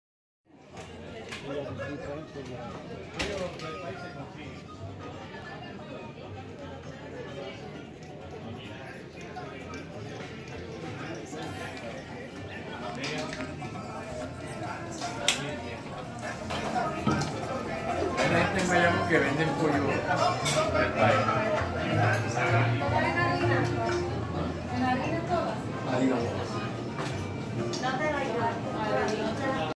Hacienda Tres Angeles Cafe in Puerto Rico
In Hacienda Tres Angeles you will be able to enjoy a wonderful experience in our coffee plantation as well as learn our agricultural practices, coffee processing and tasting. Come and live the essence of our coffee from crop to cup.

You can then enjoy a delicious cup of coffee in our Cafe Hacienda Tres Angeles, while admiring the beautiful view to the coffee field.